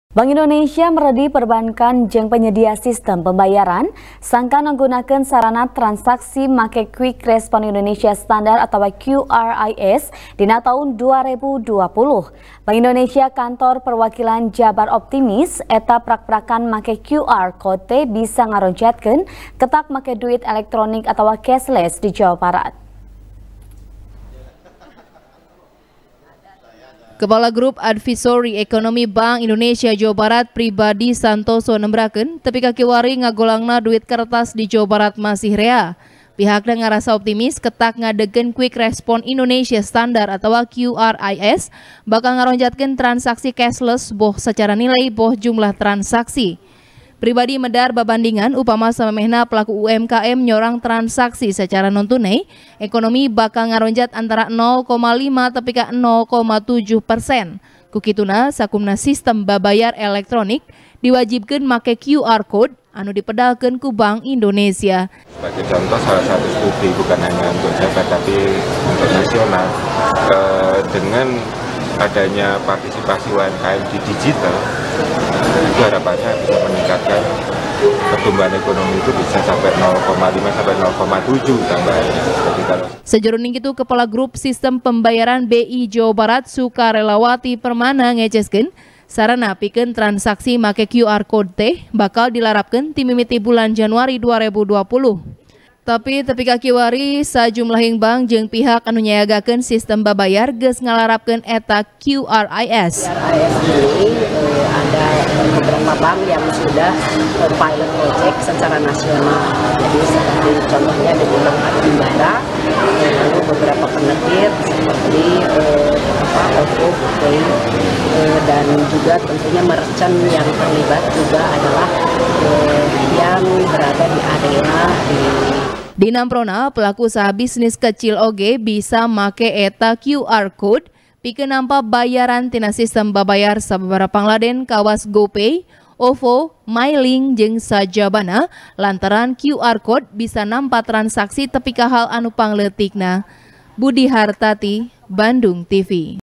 BI Yakin, Prakprakan Make Qris Ngaronjatkeun Cashless Di Jabar | TP | BANDUNGTV
TP | BANDUNGTV | ASTRI CAHYANI | Kepala grup advisory ekonomi bank Indonesia Jawa Barat, Pribadi Santoso nembrakeun, tepi ka kiwari ngagolangna duit kertas di Jabar masih rea. Pihakna ngarasa optimis, ketak ngadegkeun Quick Response Indonesia Standar atawa QRIS, bakal ngaronjatkeun transaksi Cashless boh sacara niley boh jumlah transaksi. Pribadi medar babandingan, upama samemehna pelaku UMKM nyorang transaksi sacara nontunai, ekonomi bakal ngaronjat antara nol koma lima tepi ka nol koma tujuh persen. Ku kituna, sakumna sistem babayar elektronik diwajibkeun make QR code anu dipedalkeun ku bank Indonesia.